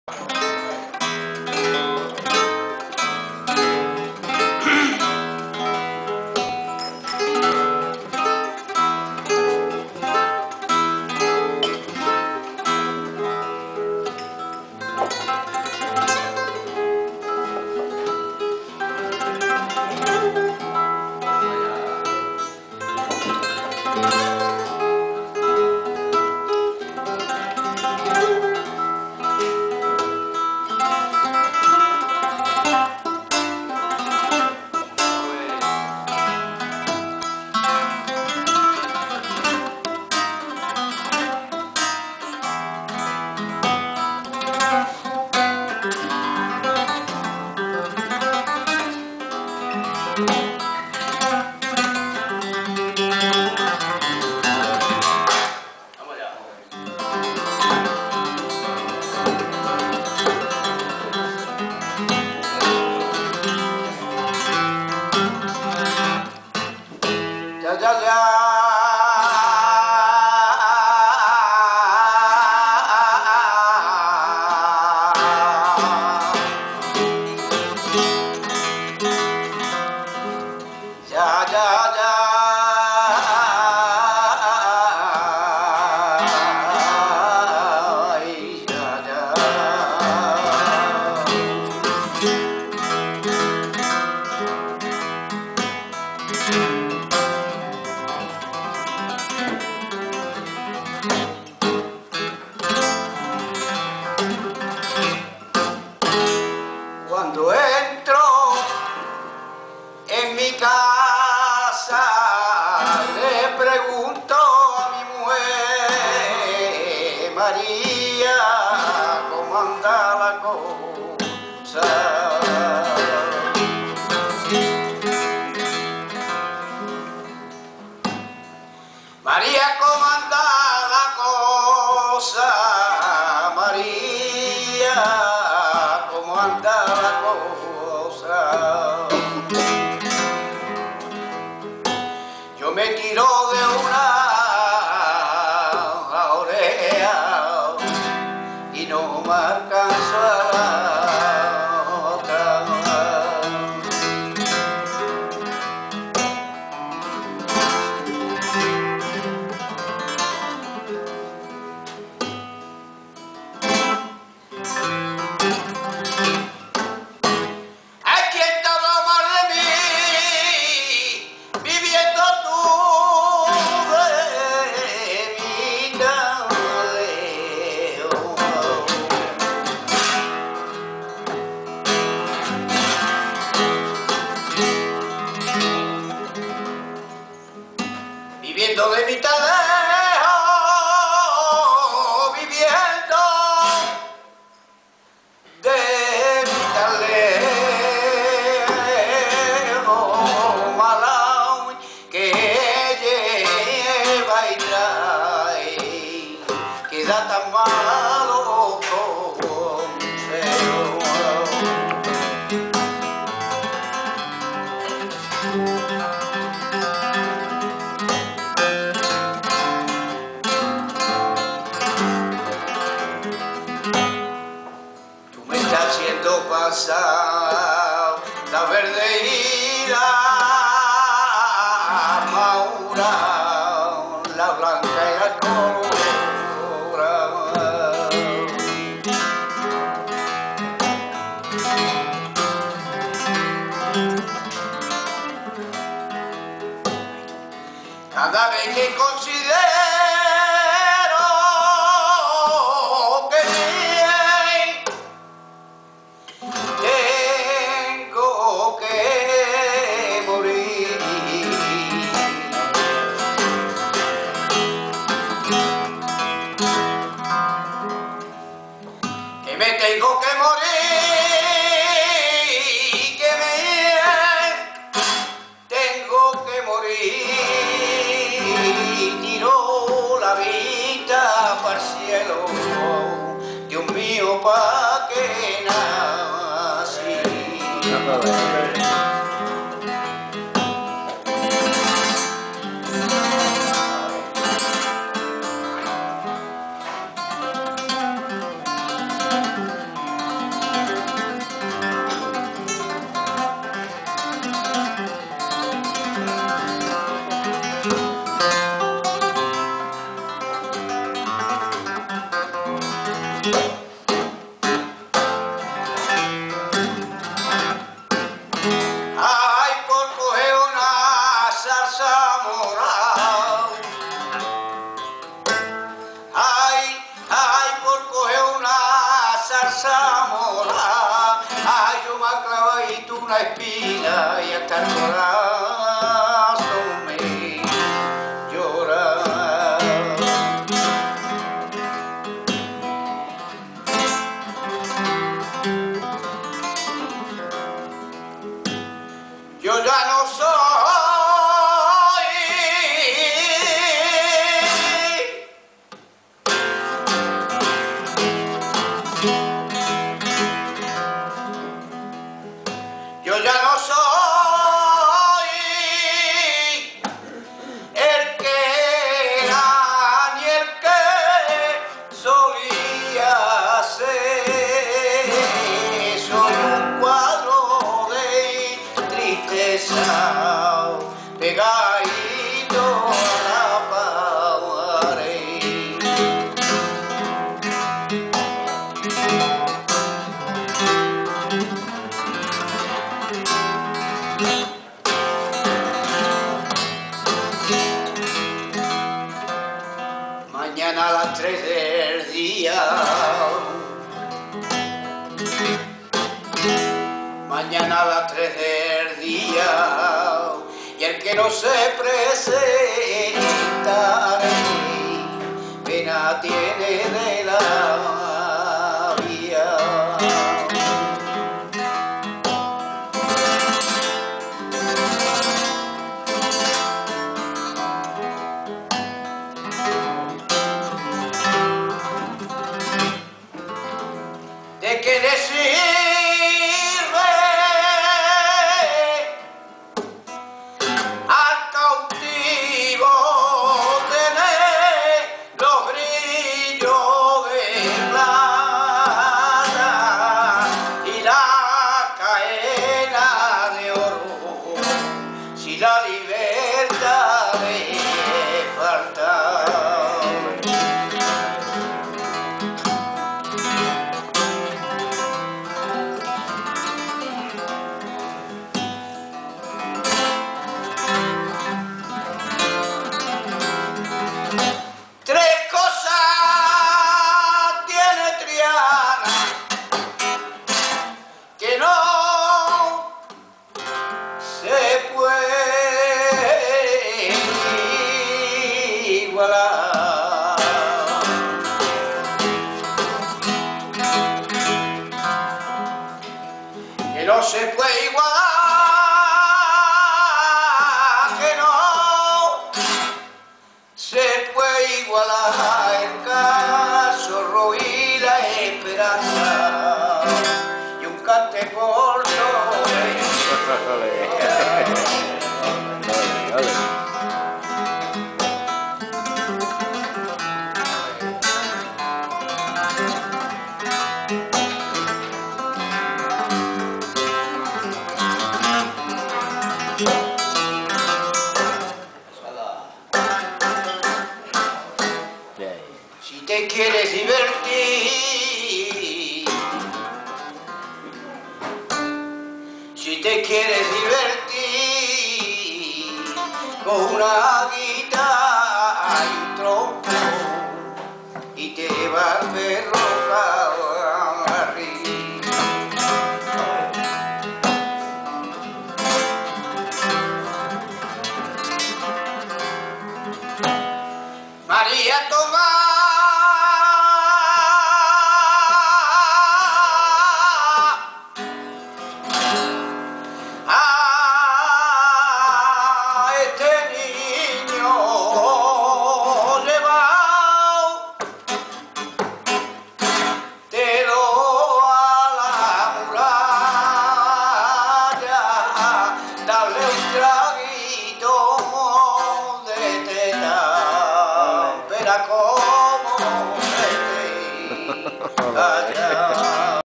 MARQUEZ EL ZAPATERO Y EDUARDO REBOLLAR BAR COCK.MPG
Aunque se ve muy oscuro, problema de mi cámara, lo que interesa es el cante, fue una velada digna de recordarla en lo más profundo del corazón.